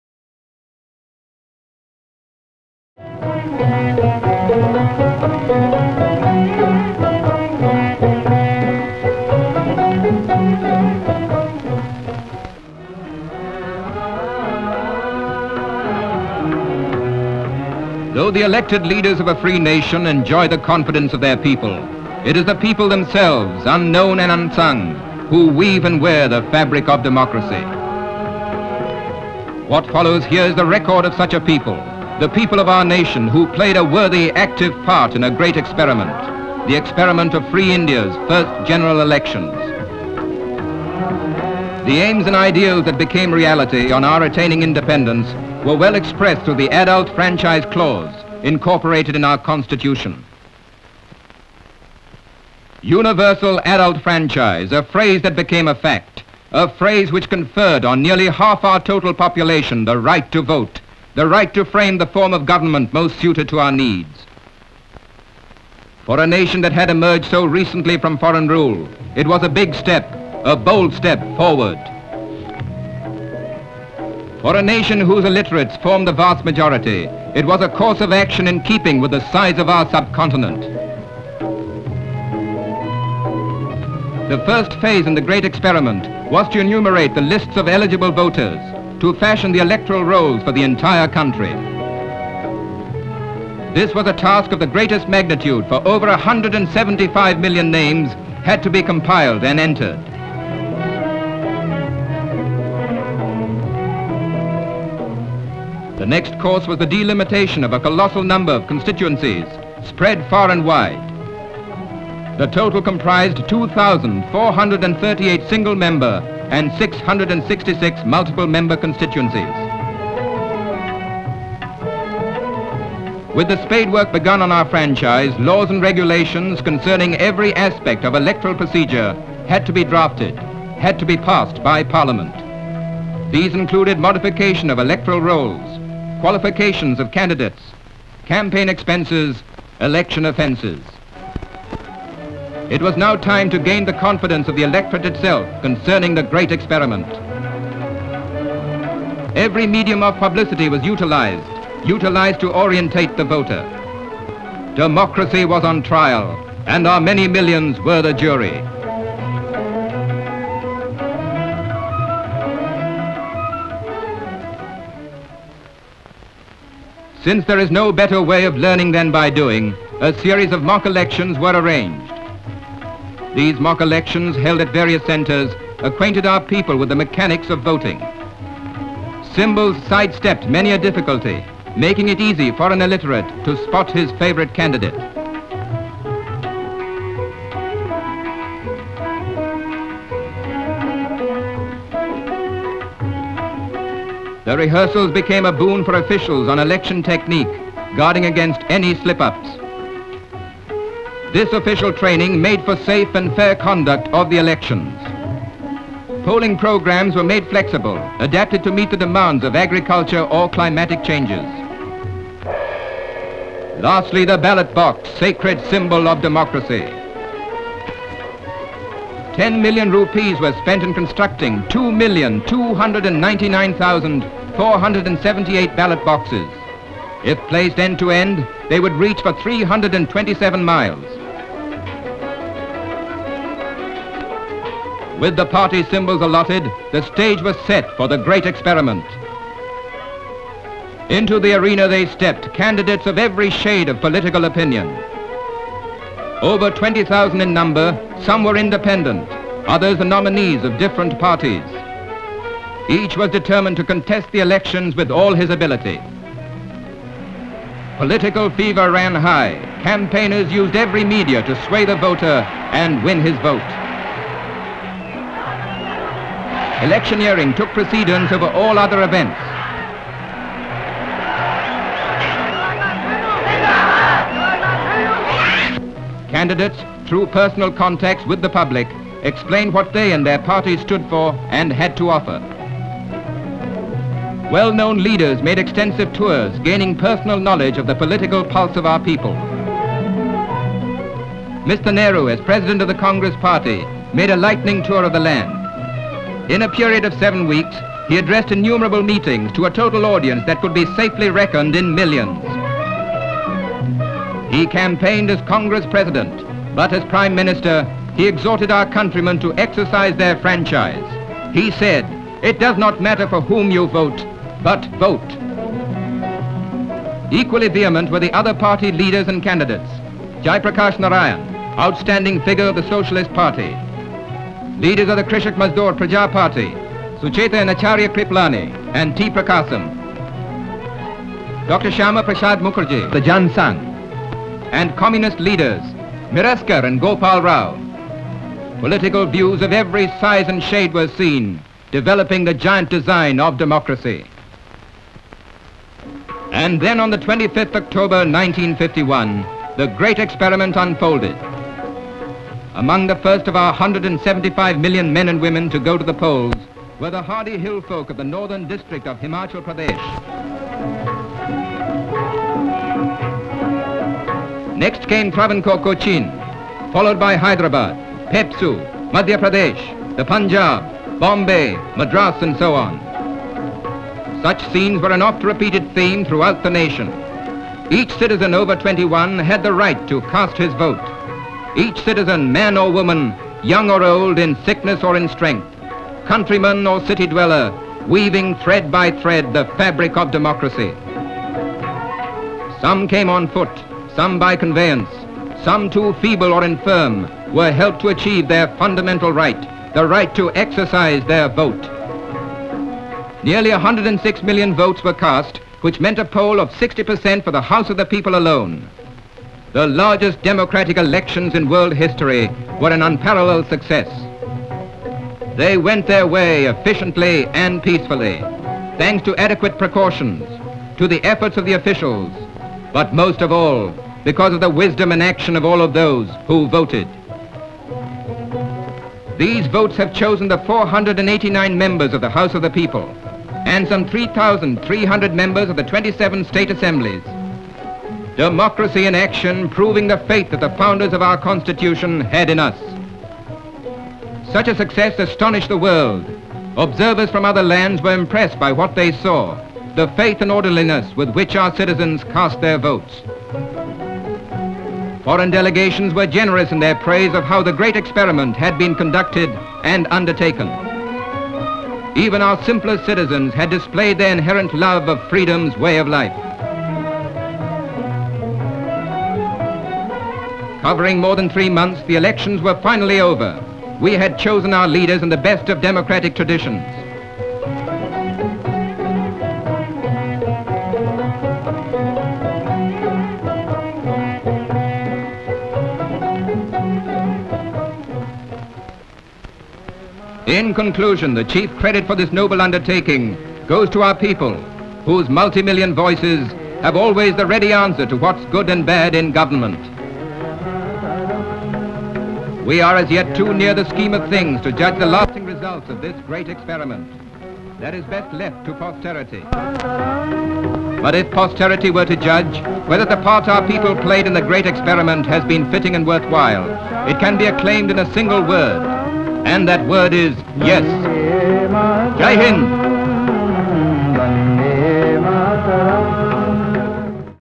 THE GREAT EXPERIMENT : 1ST GENERAL ELECTION OF INDEPENDENT INDIA
A glimpse of one of the most monumental event in the history of Indian Democracy, the first Indian Election held in 1951-52. It was the vision of our founding fathers and commitment of the electorate that made the 1st General Election a huge success in a country with such a vast vast topography, diverse cultures and varing climatic conditions.

Read the elaborative documentation of the 1st General Election 1951-52